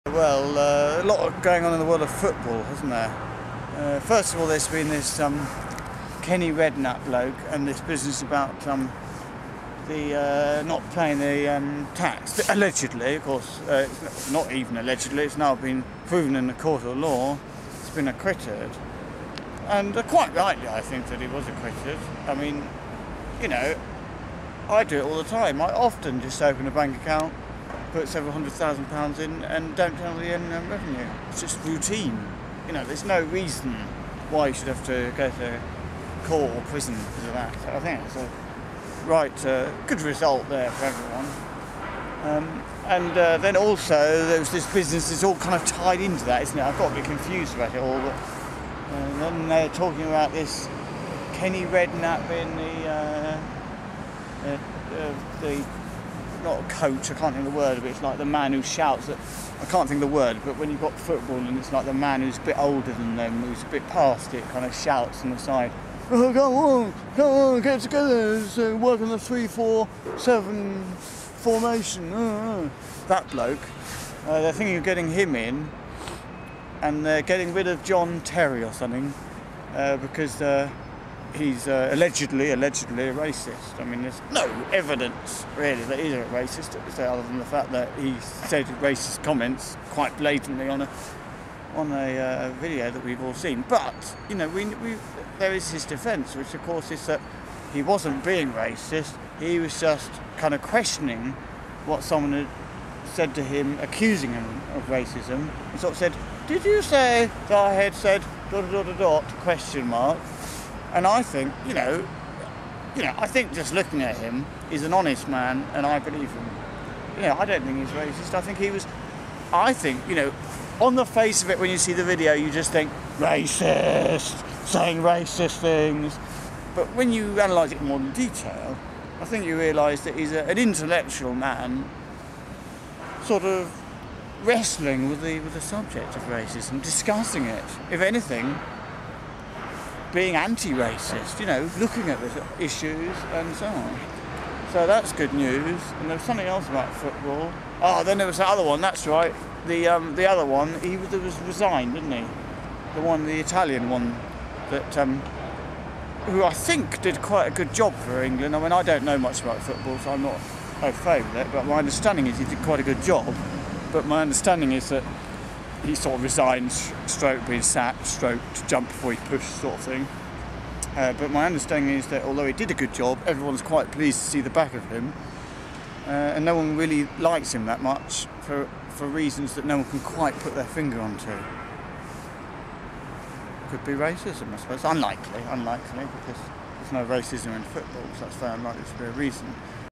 Paul Foot Video Log: Football
Here is my new video log for the second half of Fabruary. I address all the current turbulence within the whirled of football, such as Kenny Redknapp's taxes, John Terry's honesty and other matters, all of which are important as they are to do with sport, which is important, obviously.

And if you really like my comedy then join the Guild of Paul Foot Connoisseurs on my official websyte: